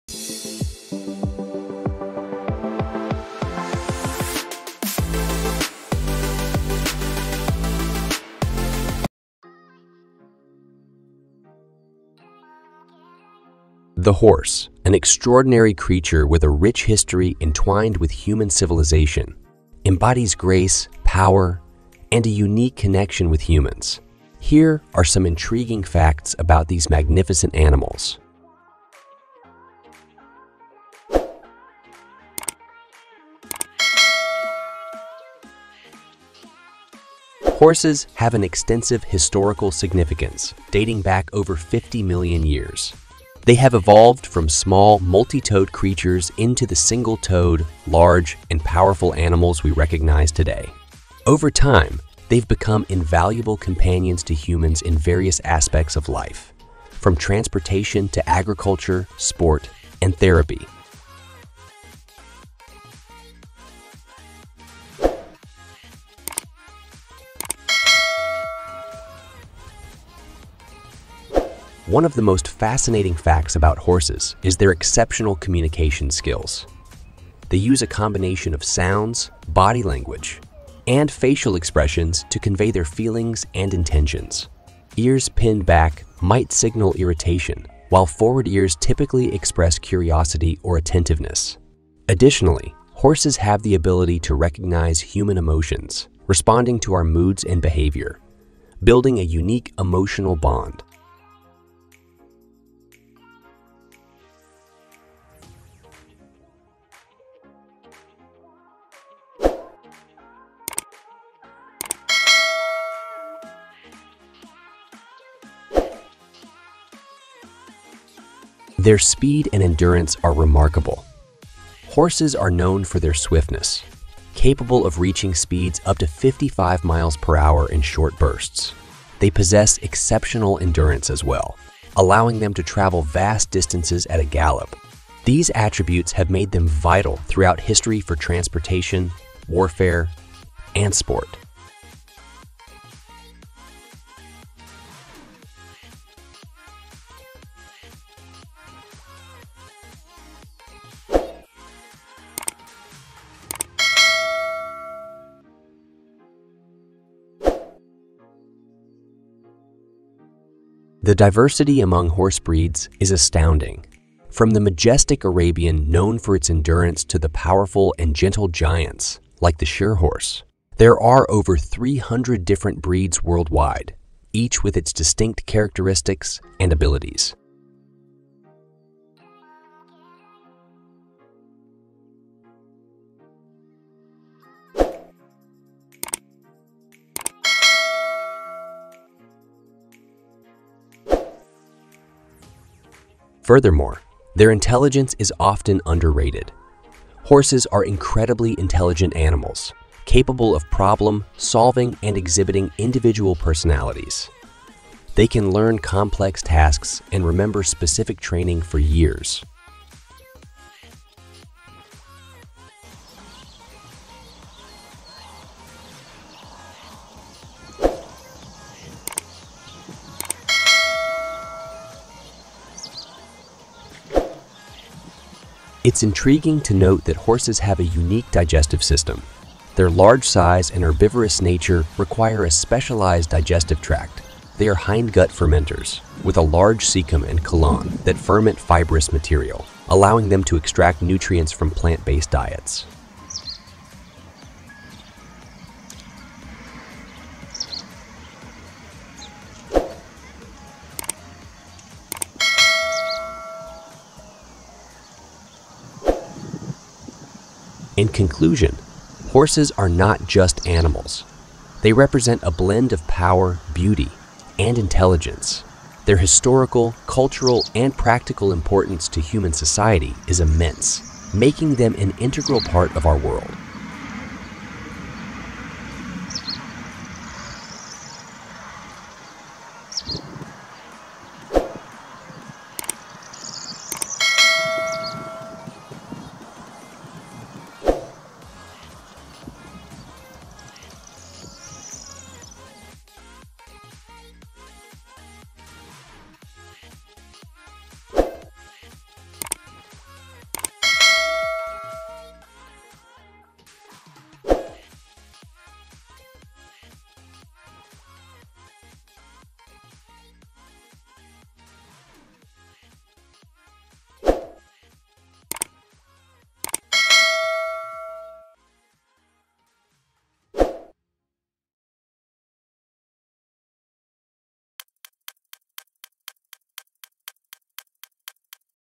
Horse vedio | අශ්වයන් ගැන බොහෝ දේ | marwari horse vedio | tranding vedio | animals lifestyle vedio
The horse, an extraordinary creature with a rich history entwined with human civilization, embodies grace, power, and a unique connection with humans. Here are some intriguing facts about these magnificent animals.

  Horses have an extensive historical significance, dating back over 50 million years. They have evolved from small multi-toed creatures into the single-toed, large, and powerful animals we recognize today. Over time, they've become invaluable companions to humans in various aspects of life, from transportation to agriculture, sport, and therapy.

  One of the most fascinating facts about horses is their exceptional communication skills. They use a combination of sounds, body language, and facial expressions to convey their feelings and intentions. Ears pinned back might signal irritation, while forward ears typically express curiosity or attentiveness. Additionally, horses have the ability to recognize human emotions, responding to our moods and behavior, building a unique emotional bond.

  Their speed and endurance are remarkable. Horses are known for their swiftness, capable of reaching speeds up to 55 miles per hour in short bursts. They possess exceptional endurance, as well, allowing them to travel vast distances at a gallop. These attributes have made them vital throughout history for transportation, warfare, and sport.

  The diversity among horse breeds is astounding. From the majestic Arabian known for its endurance to the powerful and gentle giants like the Shire horse, there are over 300 different breeds worldwide, each with its distinct characteristics and abilities.

  Furthermore, their intelligence is often underrated. Horses are incredibly intelligent animals capable of problem-solving and exhibiting individual personalities. They can learn complex tasks and remember specific training for years.

  It’s intriguing to note that horses have a unique digestive system. Their large size and herbivorous nature require a specialized digestive tract. They are hindgut fermenters, with a large cecum and colon that ferment fibrous material, allowing them to extract nutrients from plant-based diets.

  In conclusion, horses are not just animals; they represent a blend of power, beauty, and intelligence. Their historical, cultural, and practical importance to human society is immense, making them an integral part of our world.

මානව ශිෂ්ටාචාරය සමඟ බැඳී ඇති පොහොසත් ඉතිහාසයක් ඇති අසාමාන්‍ය සත්වයෙකු වන අශ්වයා, කරුණාව, බලය සහ මිනිසුන් සමඟ අද්විතීය සම්බන්ධතාවයක් මූර්තිමත් කරයි.  මෙම අපූරු සතුන් පිළිබඳ කුතුහලය දනවන කරුණු කිහිපයක් මෙන්න.

   අශ්වයන්ට වසර මිලියන 50 කට වඩා පැරණි ඓතිහාසික වැදගත්කමක් ඇත.  ඔවුන් කුඩා බහු-ඇඟිලි සහිත ජීවීන්ගේ සිට අද අප හඳුනා ගන්නා තනි ඇඟිල්ල, විශාල සහ බලවත් සතුන් බවට පරිණාමය වී ඇත.  කාලයාගේ ඇවෑමෙන්, ඔවුන් ප්‍රවාහනයේ සිට කෘෂිකර්මාන්තය, ක්‍රීඩාව සහ ප්‍රතිකාරය දක්වා ජීවිතයේ විවිධ අංශවල මිනිසුන්ට මිල කළ නොහැකි සහකරුවන් බවට පත්ව ඇත.

   අශ්වයන් පිළිබඳ වඩාත් ආකර්ෂණීය කරුණක් වන්නේ ඔවුන්ගේ සුවිශේෂී සන්නිවේදන කුසලතාවයි.  ඔවුන් තම හැඟීම් සහ අභිප්‍රායන් ප්‍රකාශ කිරීම සඳහා ශබ්ද, ශරීර භාෂාව සහ මුහුණේ ඉරියව්වල එකතුවක් භාවිතා කරයි.  කන් පිටුපසට ඇමිණීම කෝපයක් ඇති කළ හැකි අතර ඉදිරියට යන කන් සාමාන්‍යයෙන් කුතුහලය හෝ අවධානය ප්‍රකාශ කරයි.  මීට අමතරව, අශ්වයන්ට මානව හැඟීම් හඳුනා ගැනීමට, අපගේ මනෝභාවයන්ට සහ හැසිරීමට ප්‍රතිචාර දැක්වීමට, අද්විතීය චිත්තවේගීය බැඳීමක් ගොඩනැගීමට හැකියාව ඇත.

   ඔවුන්ගේ වේගය සහ විඳදරාගැනීම විශිෂ්ටයි.  කෙටි පිපිරුම් වලදී පැයට සැතපුම් 55ක් දක්වා වේගයෙන් ළඟා විය හැකි අශ්වයන් ඔවුන්ගේ වේගවත් බව සඳහා ප්‍රසිද්ධය.  ඔවුන්ට සුවිශේෂී විඳදරාගැනීමක් ඇත, එසේම ඔවුන්ට විශාල දුරක් වේගයෙන් ගමන් කිරීමට ඉඩ සලසයි.  මෙම ගුණාංග ප්‍රවාහනය, යුධ කටයුතු සහ ක්‍රීඩාව සඳහා ඉතිහාසය පුරා ඒවා වැදගත් කර ඇත.

   අශ්ව අභිජනන අතර විවිධත්වය විශ්මයජනකයි.  විඳදරාගැනීම සඳහා ප්‍රචලිත අරාබියේ සිට ෂයර් අශ්වයා වැනි බලවත් හා මෘදු යෝධයන් දක්වා ලොව පුරා විවිධ වර්ග 300 කට අධික සංඛ්‍යාවක් ඇත, ඒ සෑම එකක්ම එහි සුවිශේෂී ලක්ෂණ සහ හැකියාවන් ඇත.

   තවද, ඔවුන්ගේ බුද්ධිය බොහෝ විට අවතක්සේරු කරනු ලැබේ.  අශ්වයන් යනු ගැටළු විසඳීමට සහ පුද්ගල පෞරුෂය ප්‍රදර්ශනය කිරීමට හැකියාව ඇති ඇදහිය නොහැකි තරම් බුද්ධිමත් සතුන් ය.  ඔවුන් සංකීර්ණ කාර්යයන් ඉගෙන ගත හැකි අතර වසර ගණනාවක් නිශ්චිත පුහුණුව මතක තබා ගත හැකිය.

   අශ්වයන්ට අද්විතීය ආහාර ජීර්ණ පද්ධතියක් ඇති බව සටහන් කිරීම කුතුහලය දනවන කරුණකි.  ඔවුන්ගේ විශාල ප්රමාණය සහ ශාකභක්ෂක ස්වභාවය සඳහා විශේෂිත ආහාර ජීර්ණ පත්රිකාවක් අවශ්ය වේ.